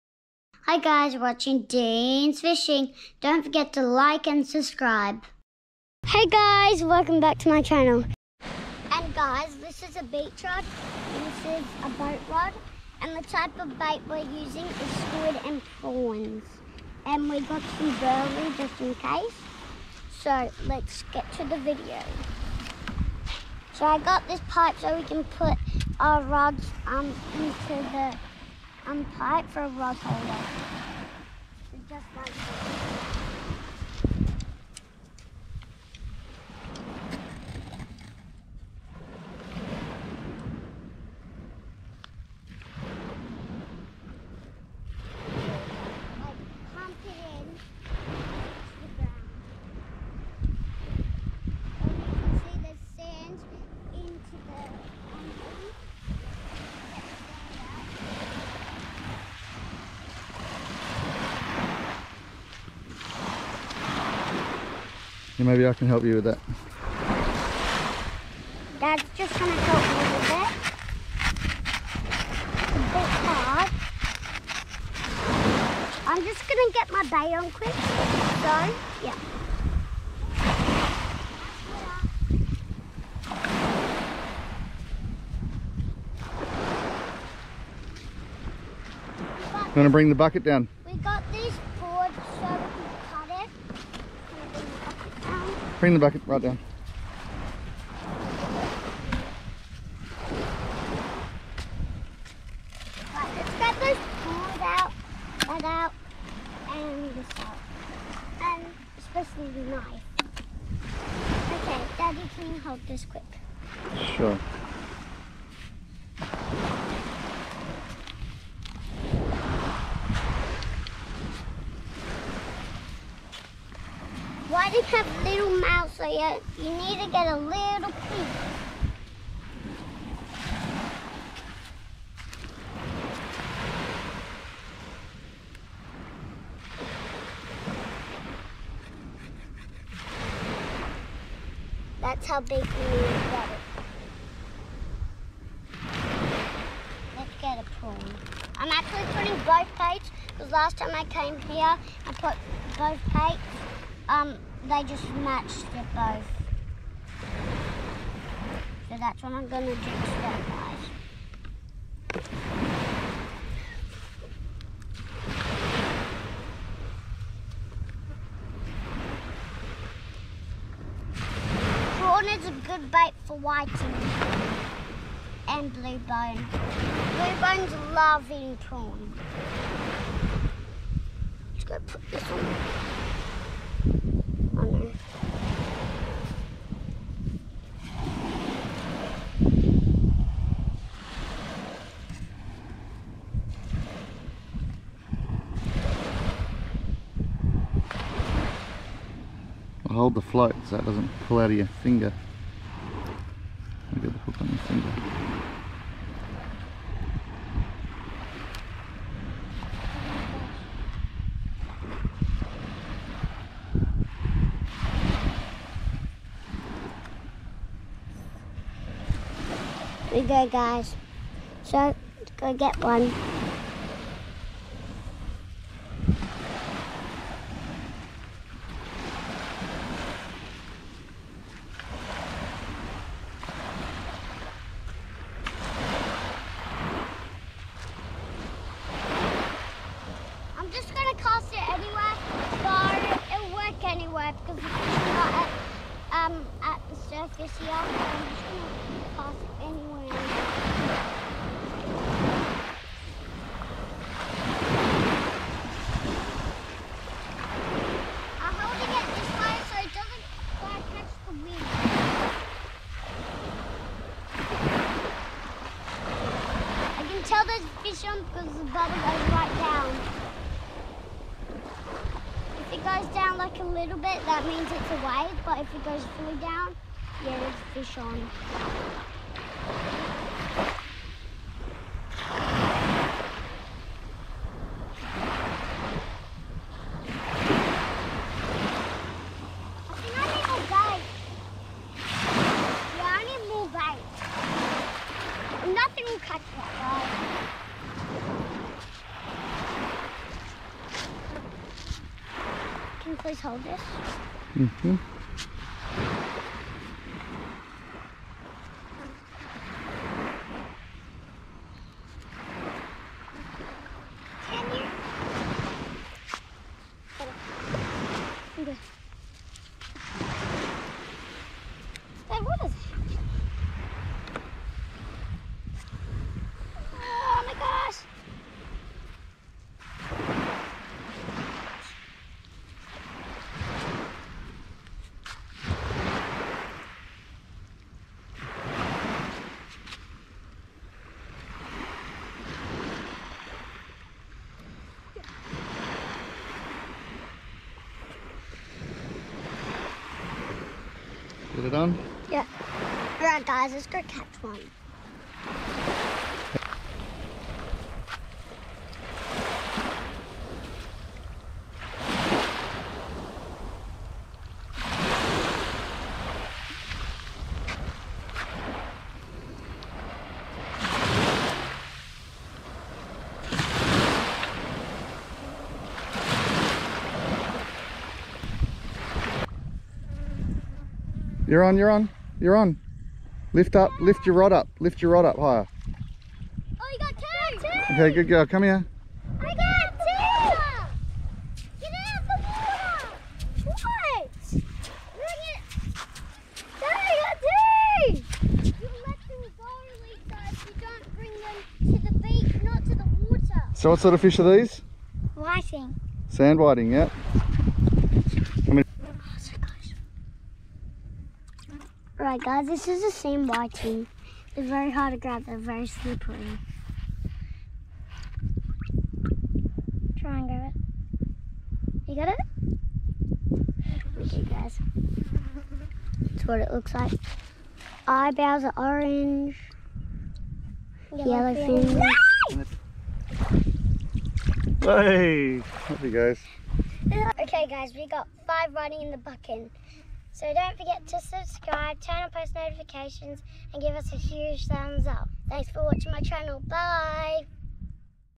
How to catch Whiting
Learn how to catch Sand Whiting with Dean's Fishing